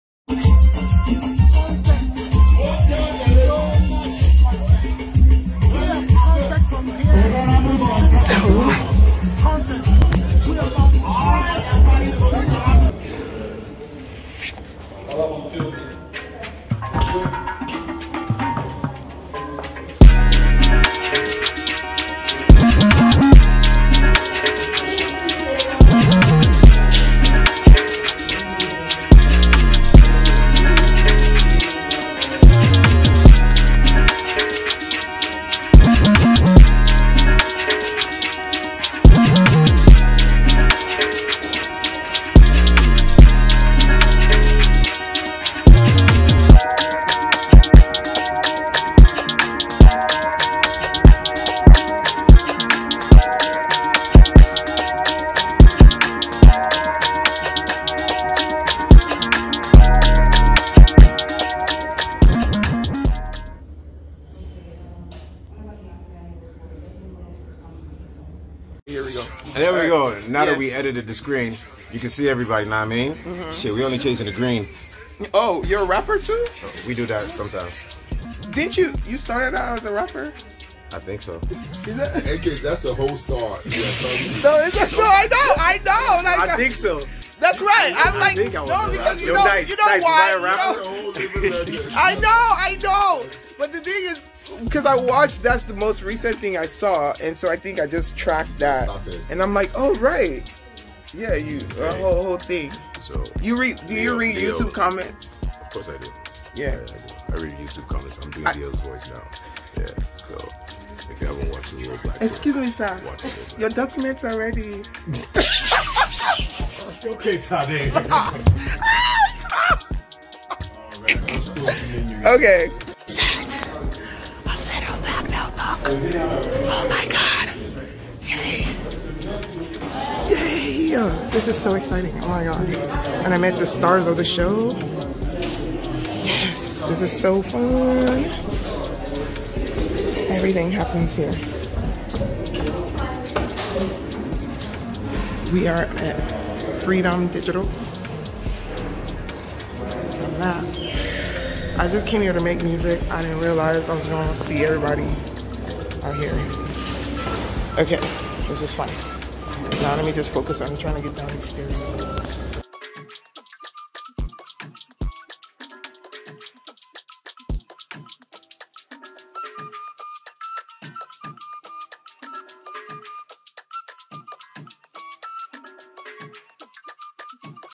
On the Set of Little Black Book with Ikechukwu & Teniola Aladese
Season 2 of Little Black Book on @tnc.africa, one of my binge-watch webseries, dropped yesterday on YouTube. So I imagine it's fitting to post this somewhat throwback pic of my nerdy self in between the stars of the show, while they were shooting szn 2.

I 'crashed' their set, and legit did a lil' "omg can I please get a pic?"  COSSS @theteniola Teniola Aladese is one my FAV Nigerian actress (she needs more leading roles in feature films please!) and little did I know that another fav Nengi Adoki @thenengers is also on season two. It's bout to be mad STEAMY, I see!

Before this my brother AO @aoworld tried to play me asking if I knew @official_ikechukwu, when I was asking him question, and low-key fangirling. like he was putting me on. Is it because Ikechukwu is forming actor now? What Ikechukwu won't remember is that back in the day I used to edit profiles and articles of him and upload his music to the online magazine Nigerian Entertainment, so yea nigga I know you a whole rapper.
YouTube.